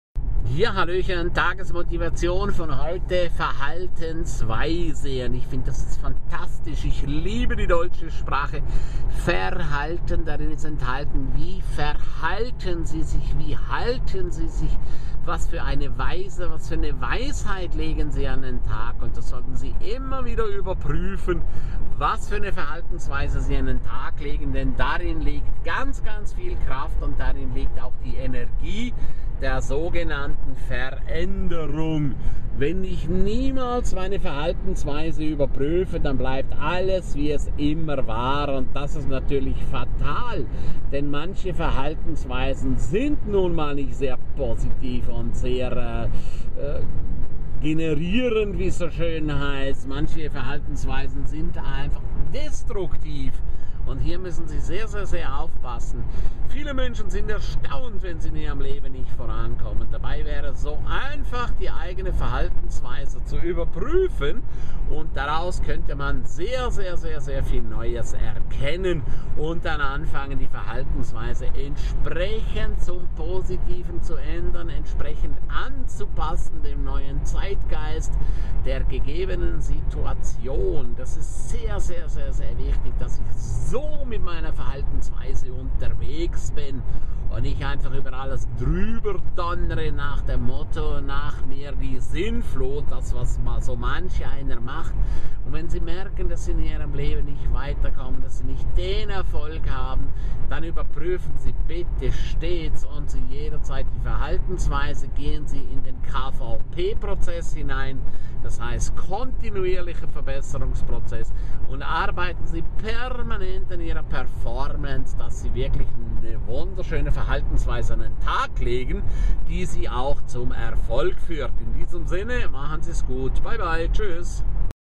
Verhaltensweise - Tagesmotivation zum Erfolg von Ernst Crameri
Welche Verhaltensweise legen Sie an den Tag? Wie gehen Sie mit Ihrem Erfolg um? Wohin geht Ihre weitere Lebensreise?